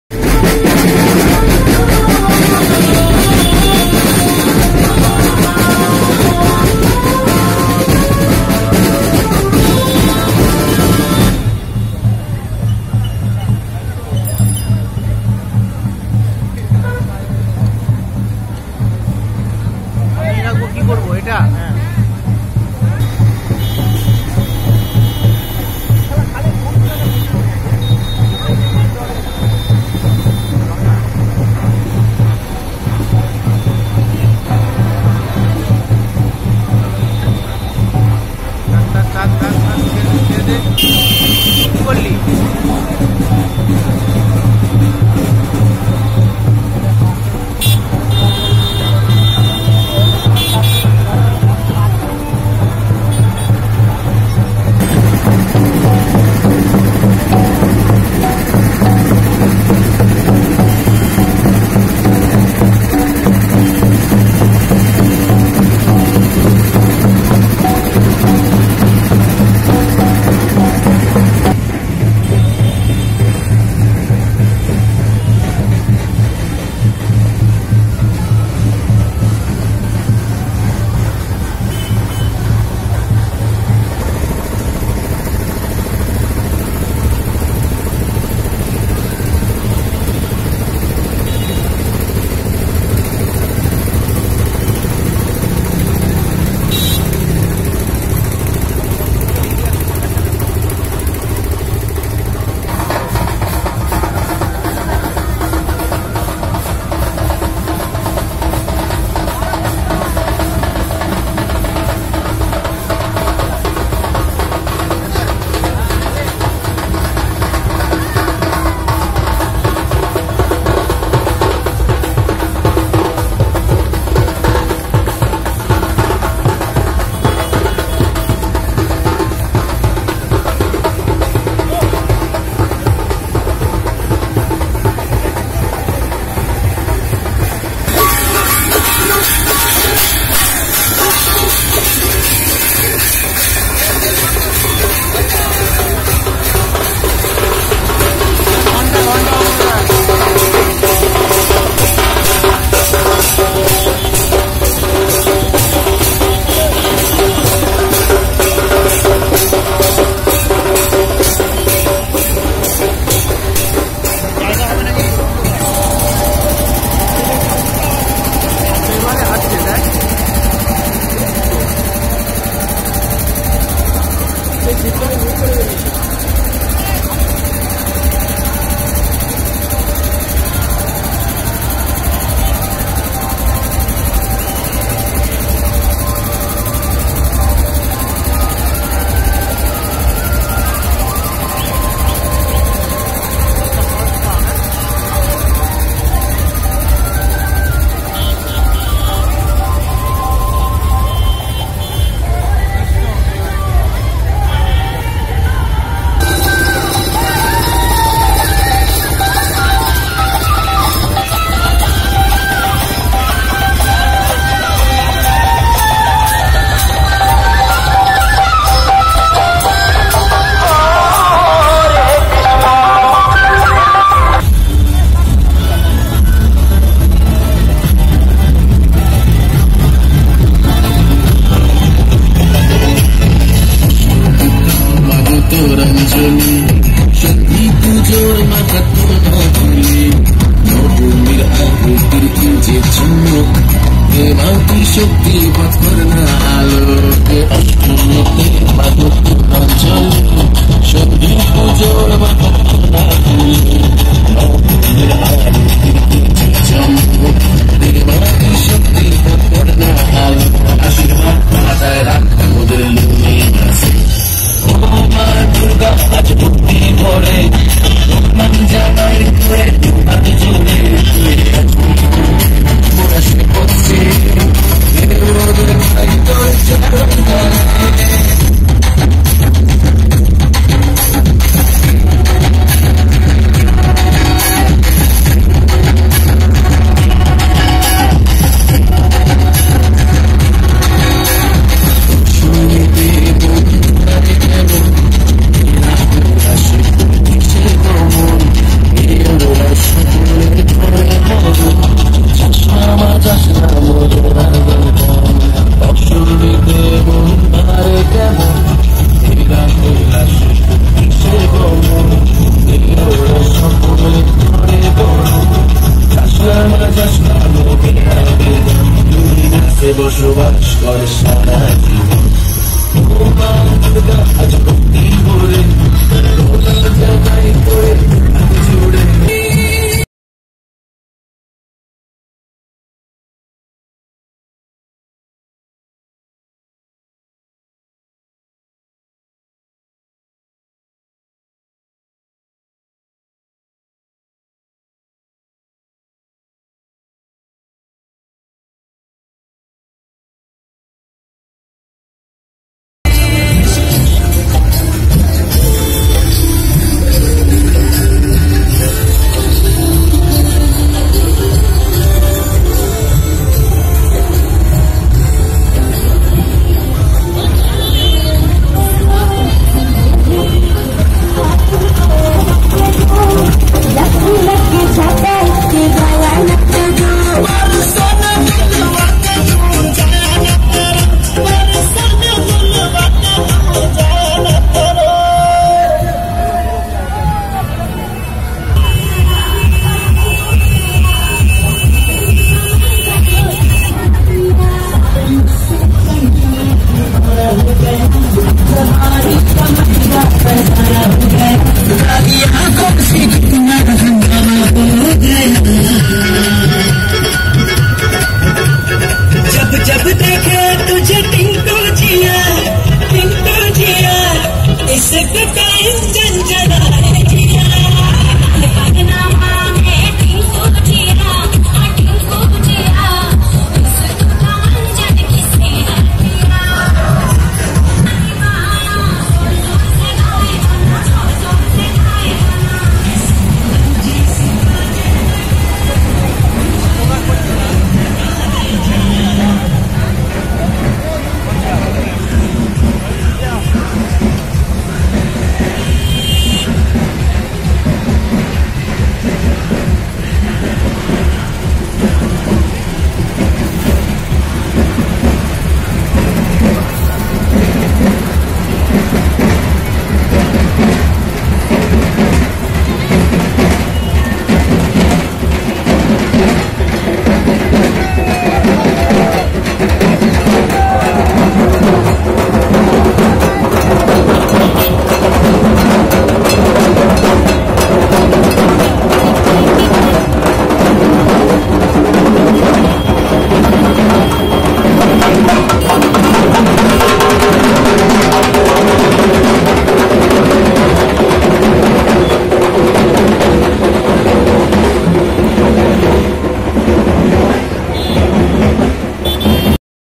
Durga Puja Carnival, Kalyani, Nadia, I.T.I More (2019)
The Durga Puja Carnival of 2019 in Kalyani, Nadia, West Bengal. 2020 features a Stay-at-Home Durga puja reliving the memories of the past on our TV, so this small token to share with all of you. Good day!!!

Shot using Xiaomi Y2 in 1080p@60FPS